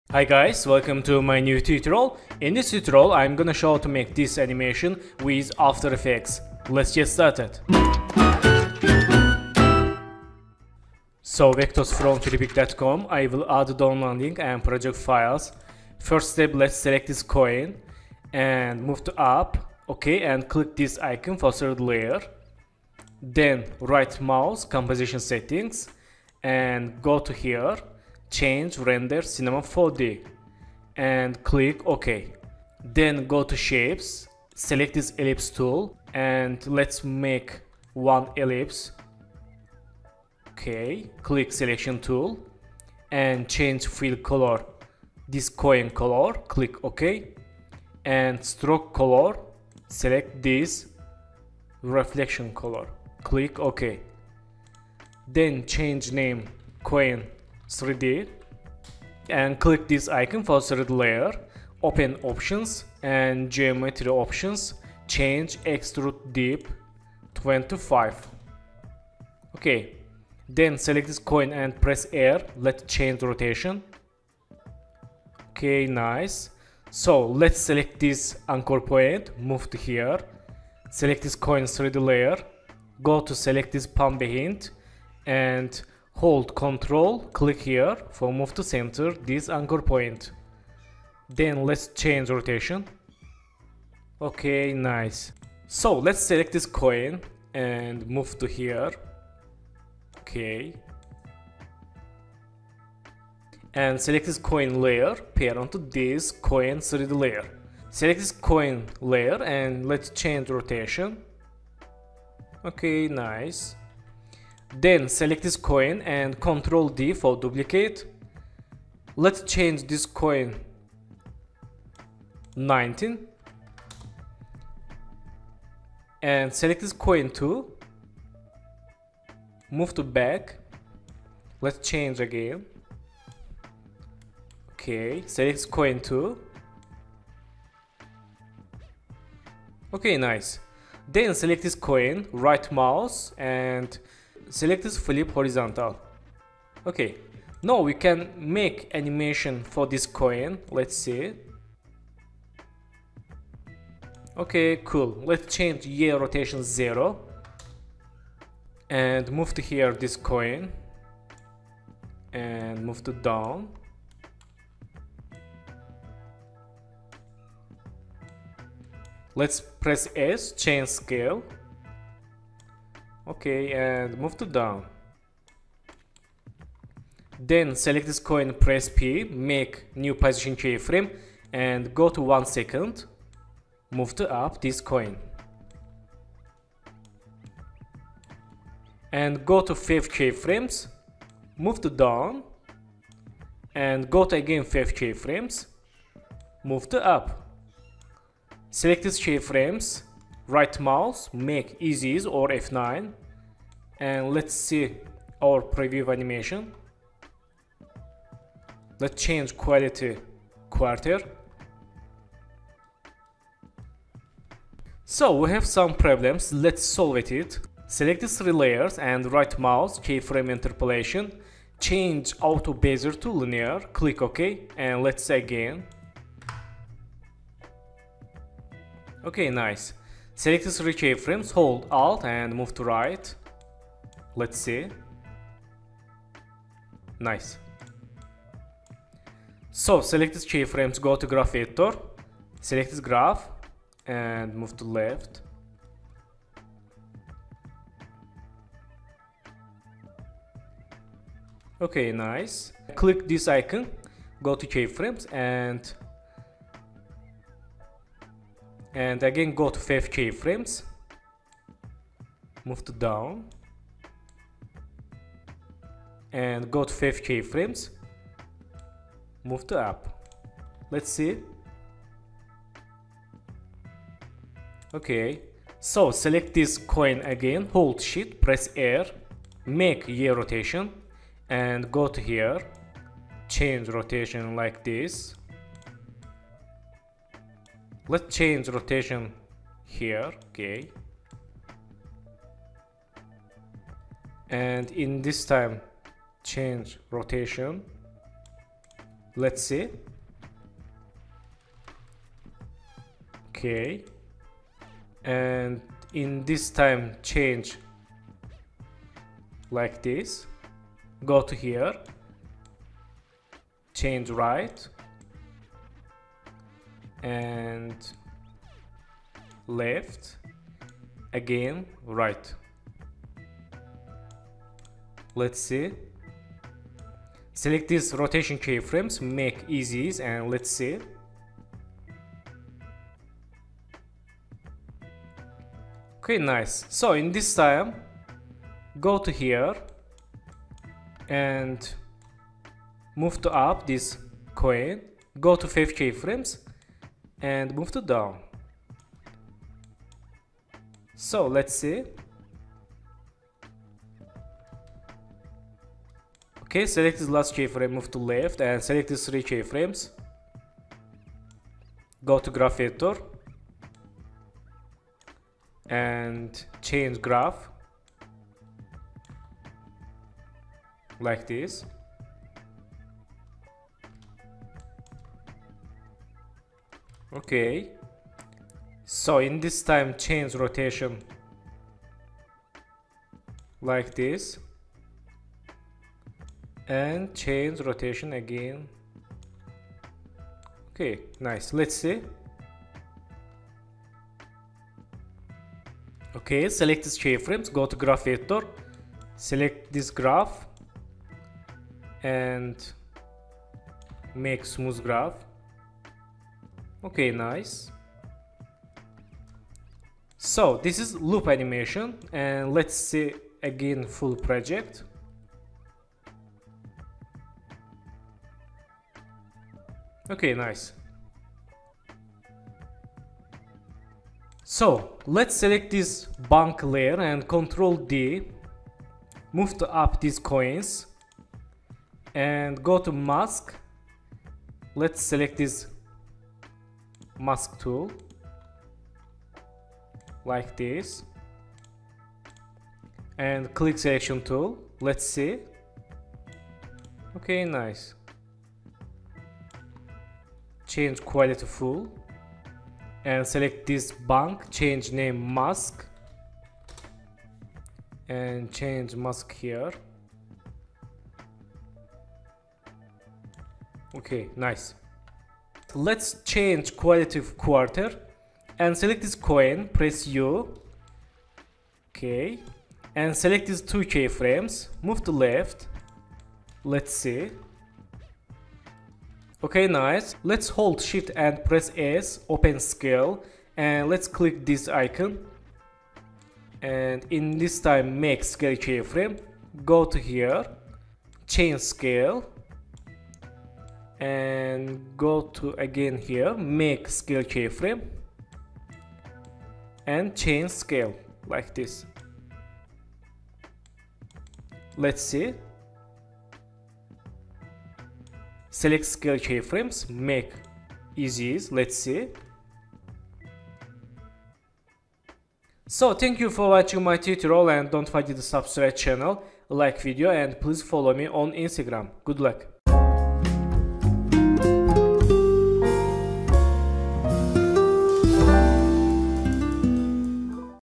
3D Coin Loop Animation in After Effects Tutorial
Hi guys, welcome to my new After Effects tutorial. In this tutorial i will show how to create 3d coin loop animation with After Effects. Let's get started!